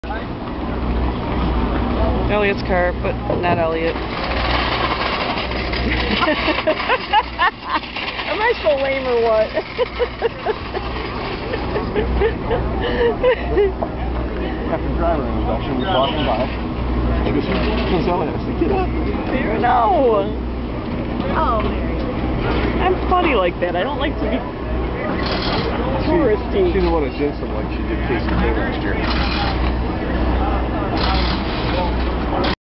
Darlington 2011 Nationwide Series post-race in the garage area
"Elliott's car, but no Elliott"!